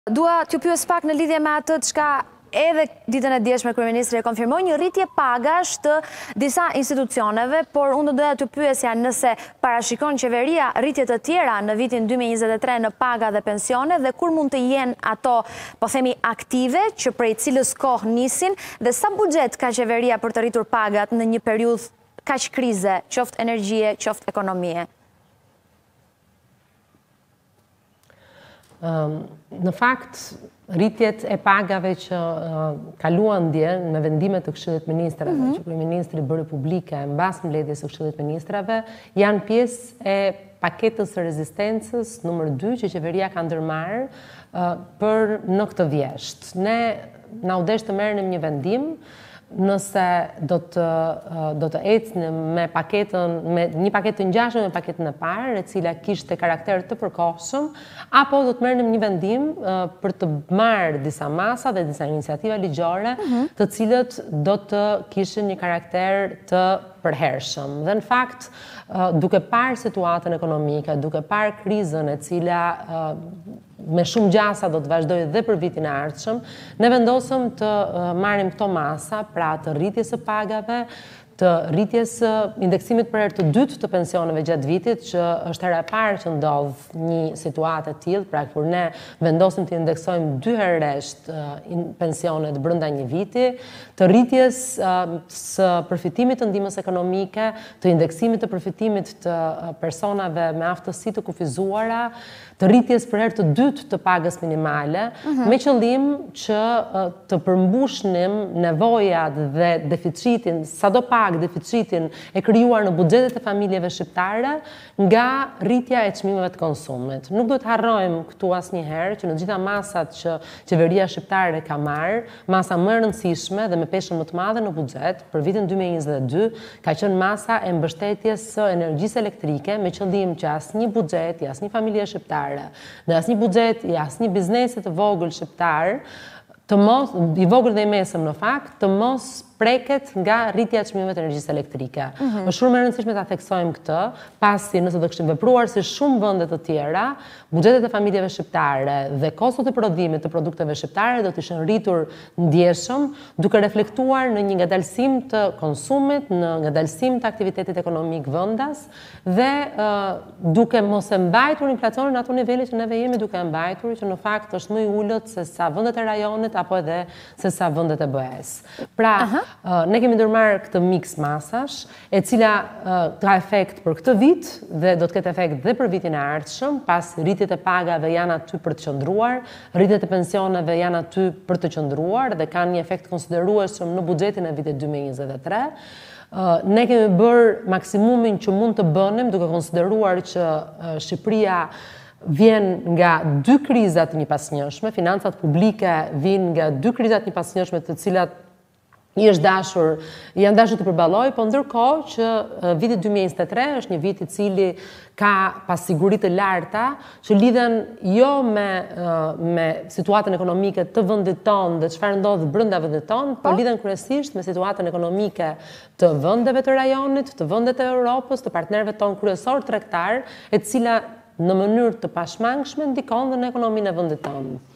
Delina Ibrahimaj: Rritja e pagave, pjesë e paketës së dytë të rezistencës sociale
Ministrja e Financave dhe Ekonomisë, Delina Ibrahimaj deklaroi se rritja e pagave për disa kategori, është pjesë e paketës së dytë të rezistencës sociale.

Në një intervistë për “Euronews Albania”, Ibrahimaj u shpreh se ky vendim u mor, për shkak se kriza ekonomike pritet që të vazhdojë edhe vitin e ardhshëm.

"Rritjet e pagave që kaluan dje me vendim të Këshillit të Ministrave, janë pjesë e paketës së rezistencës numër 2, që qeveria ka ndërmarrë në këtë vjeshtë. Na u desh të merrnim një vendim, nëse do të ecnim me një paketë të ngjashme me paketën e parë, e cila kishte karakter të përkohshëm, apo do të merrnim disa masa apo iniciativa ligjore që do kishin karakter të përhershëm. Duke parë situatën ekonomike, që me shumë gjasa do vazhdojë edhe për vitin e ardhshëm, ne vendosëm të marrim këto masa", theksoi ajo.

Këshilli i Ministrave vendosi ditën e djeshme rritje të pagave për punonjësit në Policinë e Shtetit, Policinë e Bugjeve, zjarrfikëset dhe administratën publike.